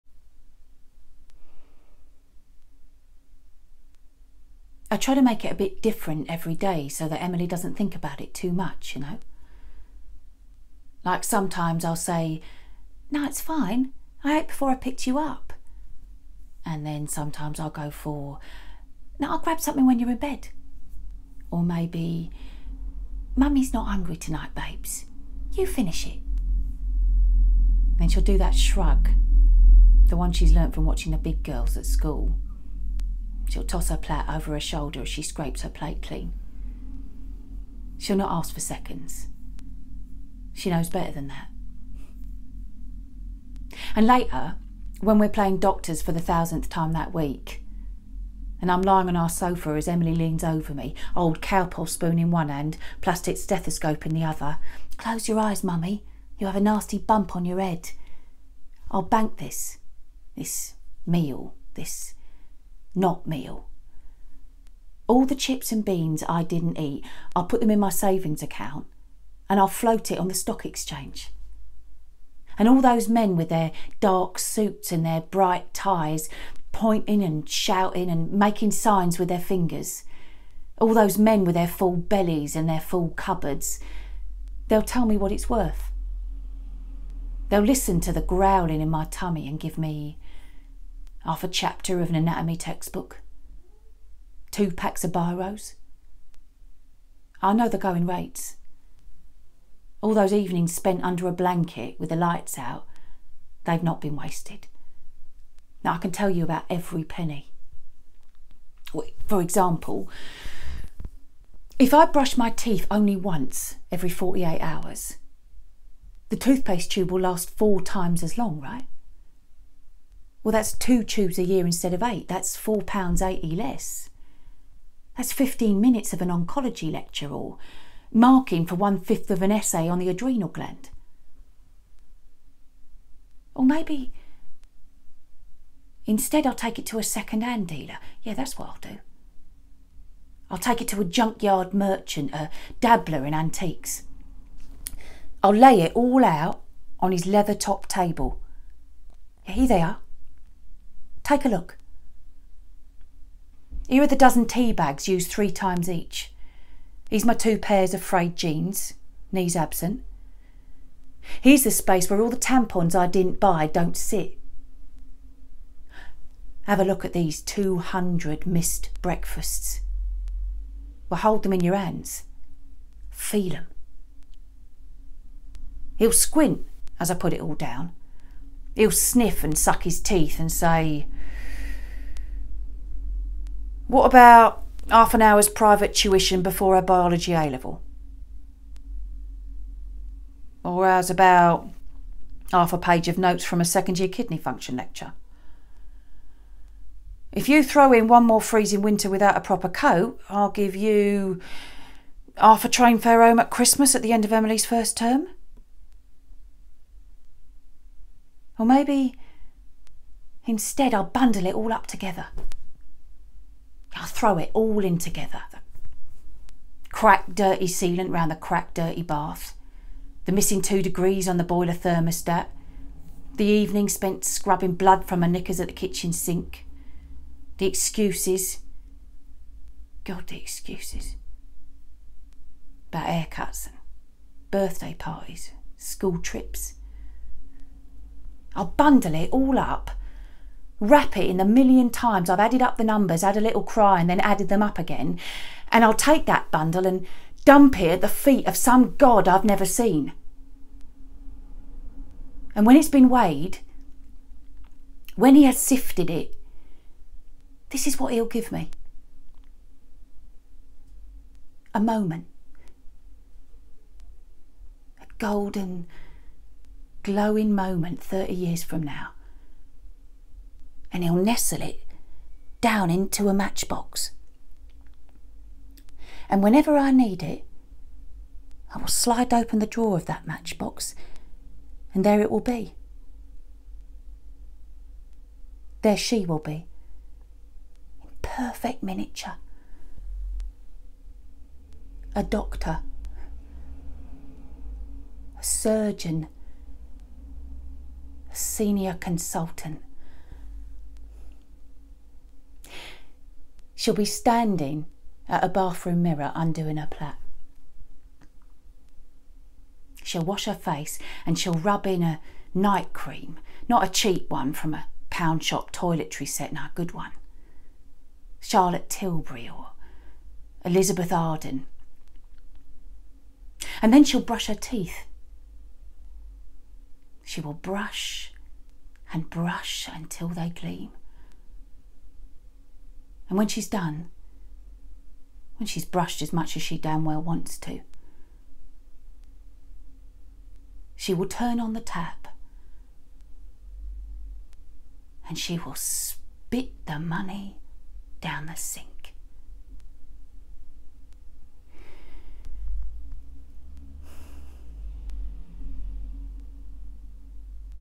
Exchange Rate
My first attempt at a monologue script. Performed by the wonderful Emma Stansfield (Coronation Street, Jamie Johnson and many other things!). This was adapted from my flash fiction story, EXCHANGE RATE, which was selected for the BIFFY50 2019 (the top 50 pieces of British and Irish Flash Fiction of the previous year.)